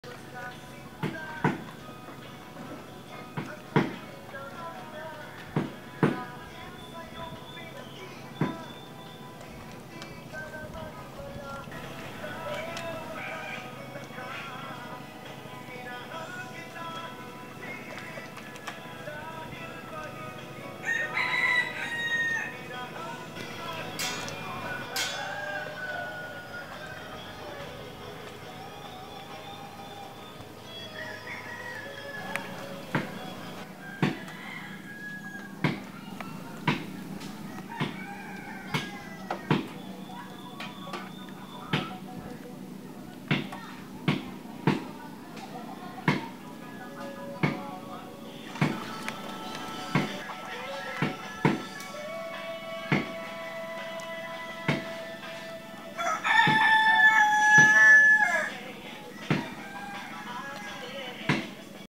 Painting The KDDJ Boat In The Water
My Sister Nancy and My brother -in-law is the owner of KDDJ boats. We re-painted this boat to make the KDDJ boats Identical in colors.. 3 of us Poldo, Robert and myself finished it in just one day..